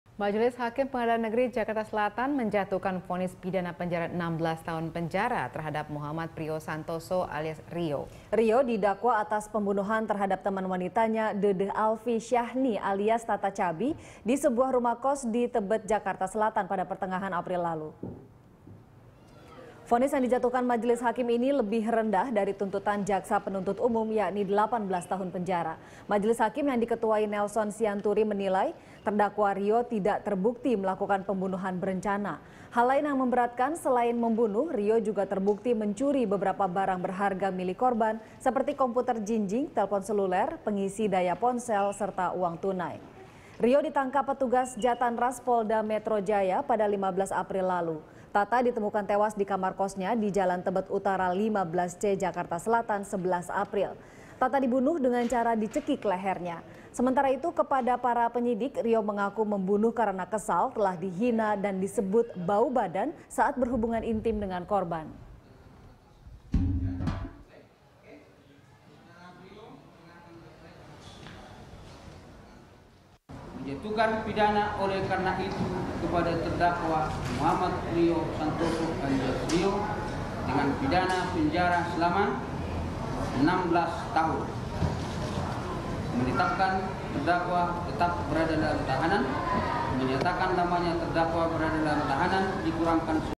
Pembunuh Tata Chubby Divonis 16 Tahun Penjara
Majelis Hakim Pengadilan Negeri Jakarta Selatan menjatuhkan vonis pidana penjara 16 tahun penjara terhadap Muhammad Prio Santoso alias Rio, Senin (30/11/2015).

Facebook.com/BeritaSatuTV
Youtube.com/BeritaSatu